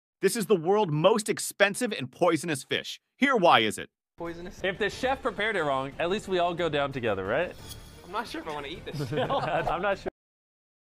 the most expensive and poisonous fish
The pufferfish, also known as fugu in Japan, is infamous for its potent neurotoxin called tetrodotoxin, found in its organs, especially the liver and ovaries. Consumption of improperly prepared pufferfish can lead to paralysis, respiratory failure, and death. Despite its toxicity, some cultures consider properly prepared fugu a delicacy, and chefs undergo rigorous training to ensure safe culinary practices.

If ,₹MrBeast were to attempt consuming the pufferfish or any highly poisonous fish, it would be an incredibly risky and dangerous endeavor. Pufferfish contain potent toxins that can lead to severe health complications and even death if not prepared and handled by experts. Such a stunt would be strongly discouraged due to the life-threatening nature of the toxins present in these fish.